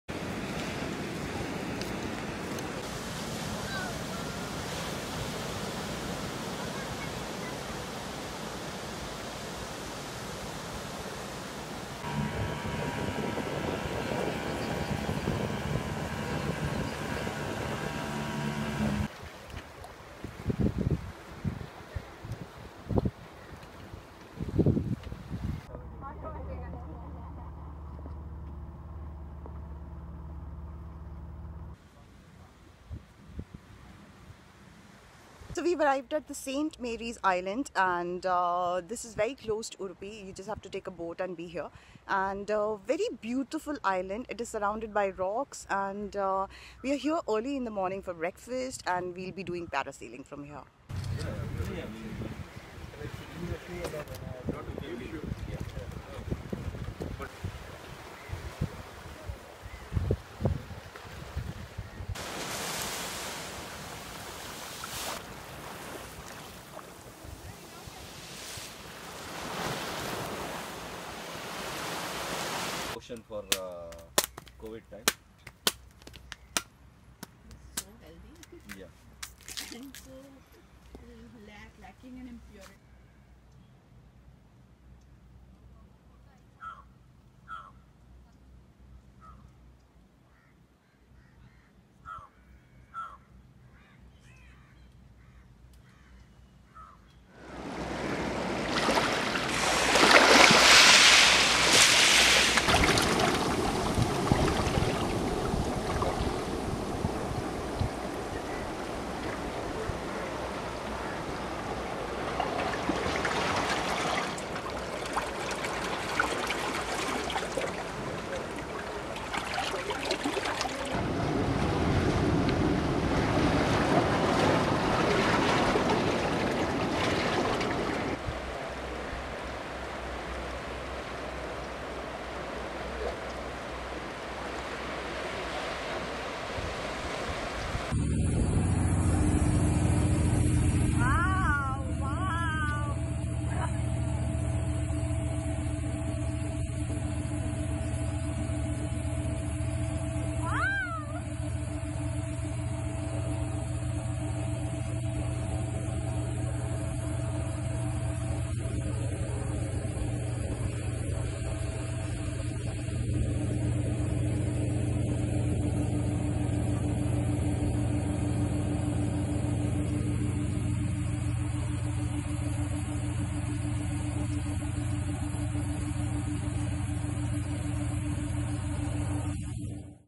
St Mary's Island Malpe Beach
When I took a trip to St Mary's Island near Malpe Beach in March 2021!